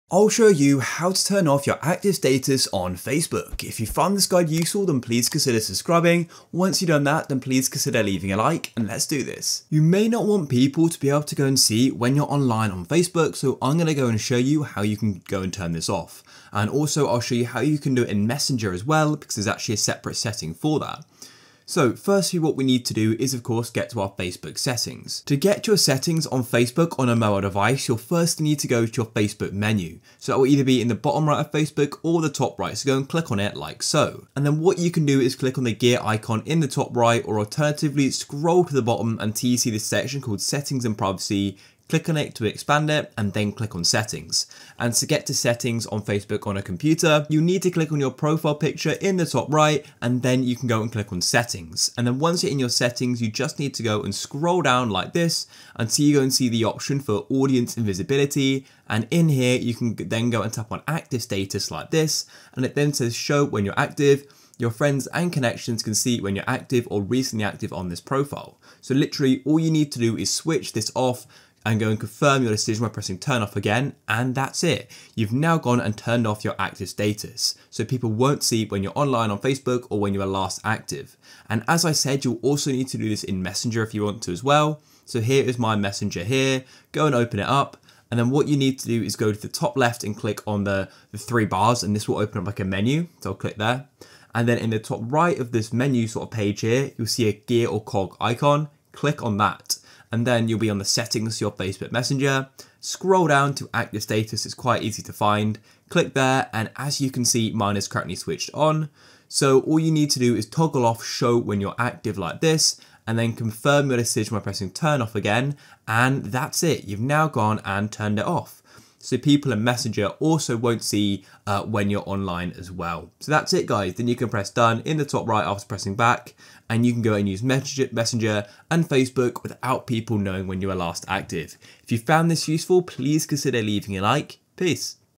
How To Turn Off Your Active Status On Facebook - Full Guide
I show you how to turn off your active status on facebook and how to hide active now on facebook in this video.

GuideRealm